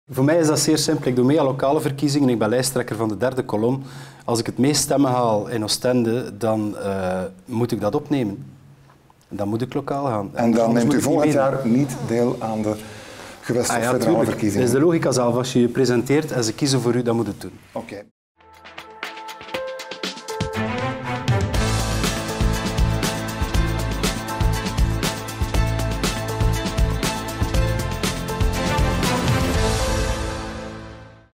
spraakmakers: "Dan moet ik lokaal gaan"- John Crombez
Joris Vanderpoorten wil in de interviewreeks 'Gemeente 18'  van sp.a-voorzitter John Crombez weten hoe hij in de toekomst zijn politiek carrière ziet nu het mandaat van burgemeester of schepen binnen zijn partij niet langer gecumuleerd mag worden met een zetel in het parlement.